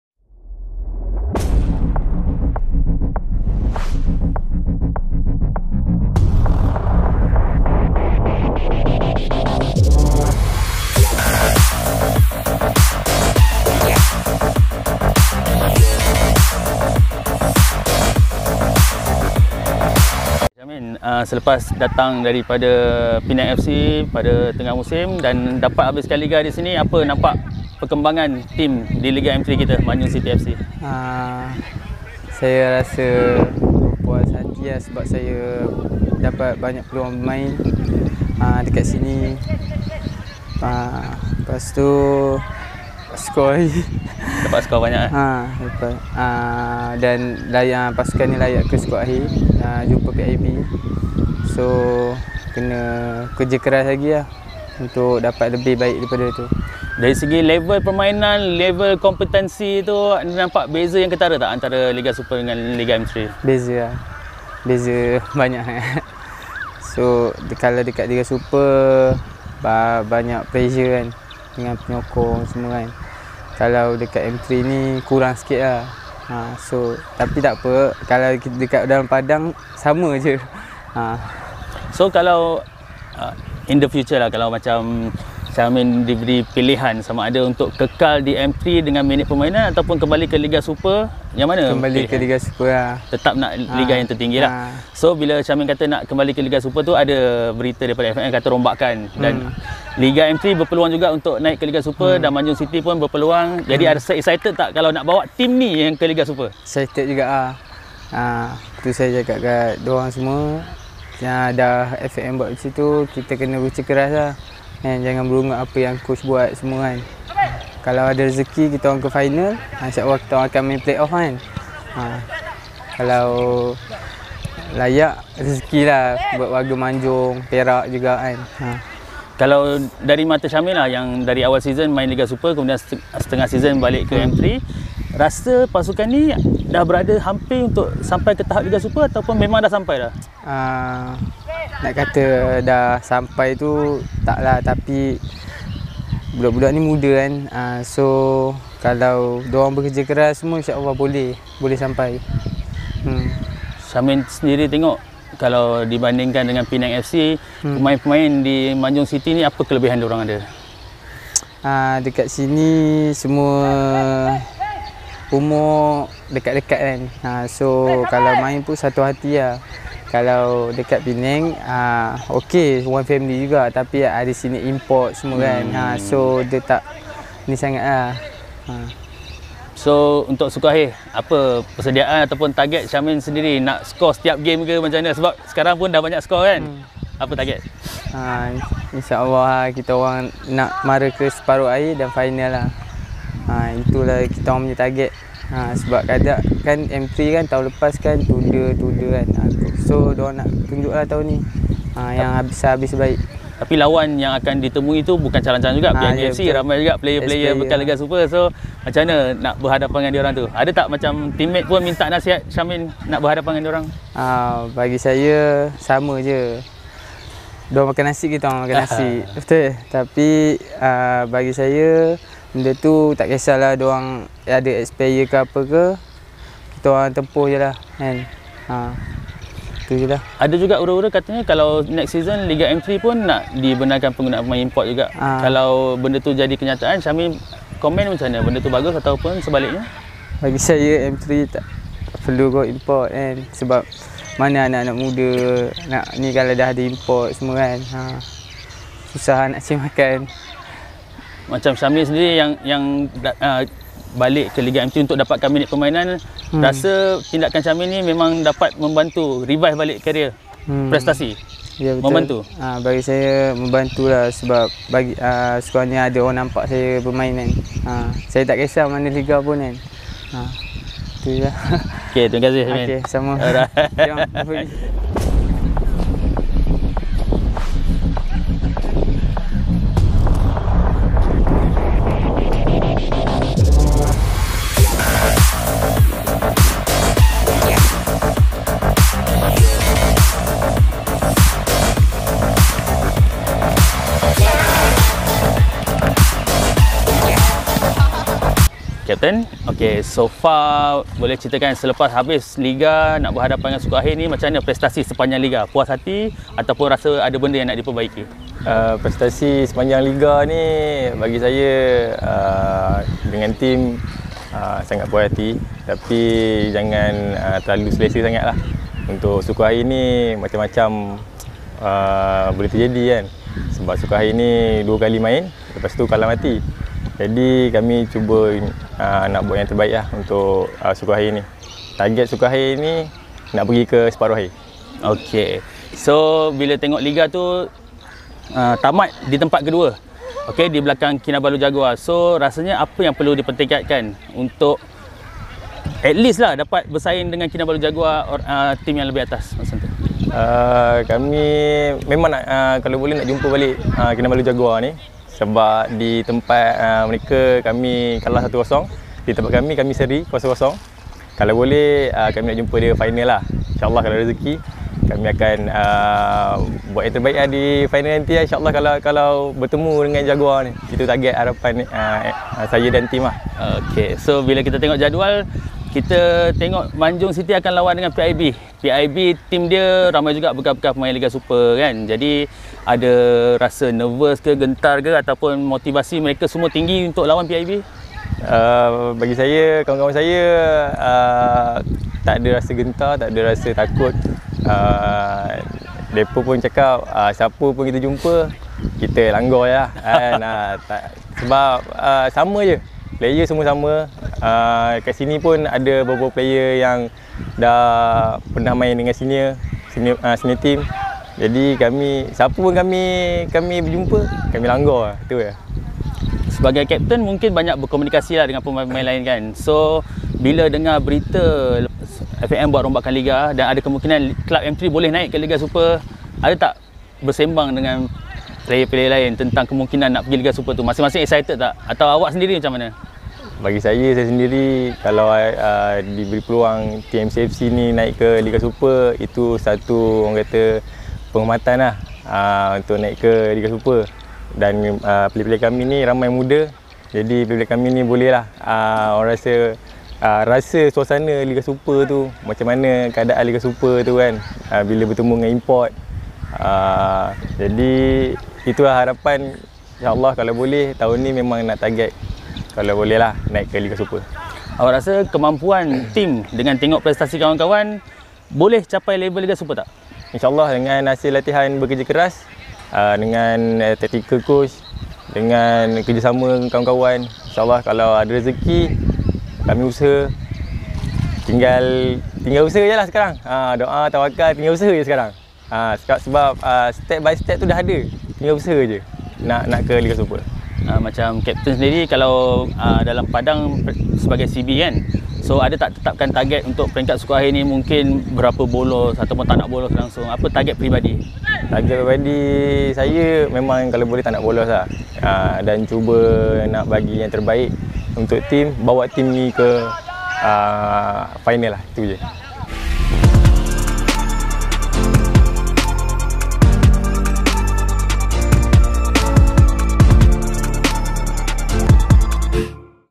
"Walaupun PIB FC ramai ex player, kami akan tempuh je! Mereka makan nasi, kami pun makan nasi!"
Temubual eksklusif bersama pemain midfield dan kapten Manjung City FC iaitu Syamin Baharuddin & Faizzuwan Dorahim mengenai persiapan menghadapi PIB FC di suku akhir Liga M3